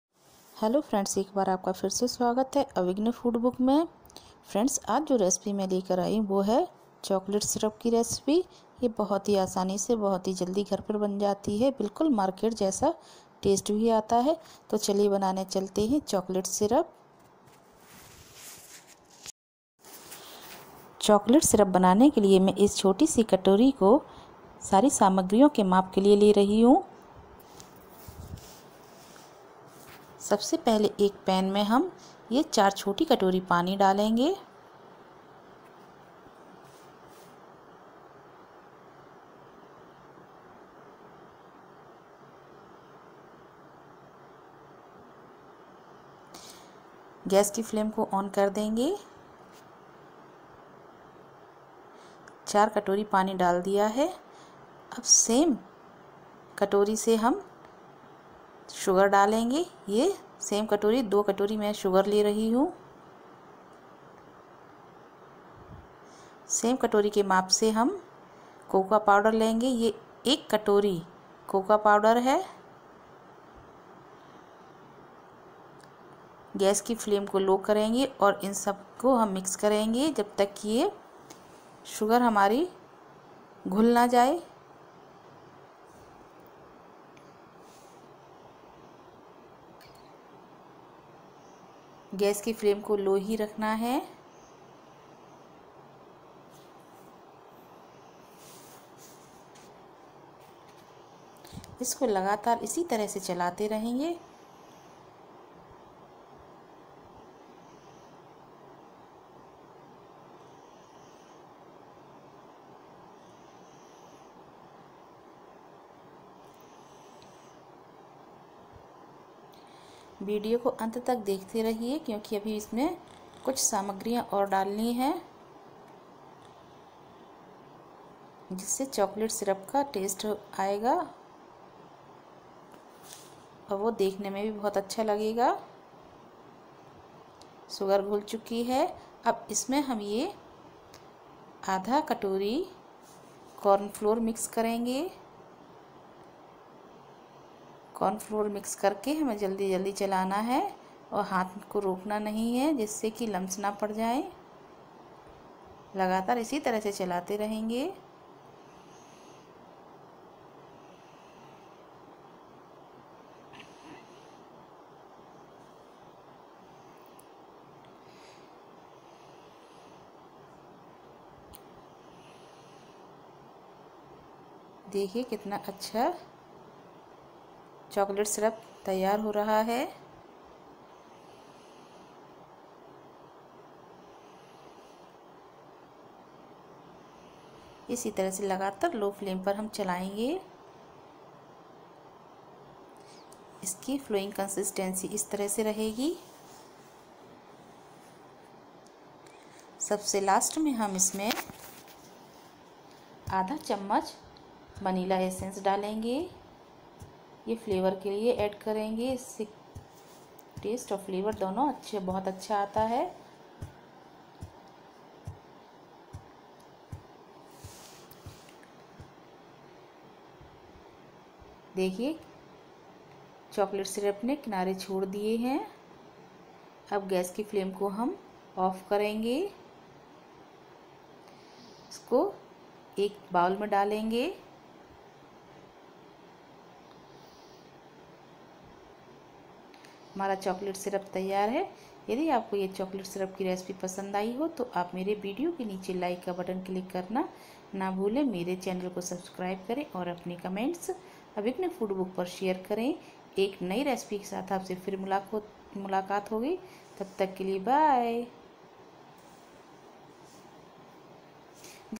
The Best Chocolate Syrup Recipe with 3 Ingredients | Homemade Chocolate Syrup made by cocoa powder
Hello friends.
welcome to Avighna food book. Today I am going to share Chocolate Syrup recipe.]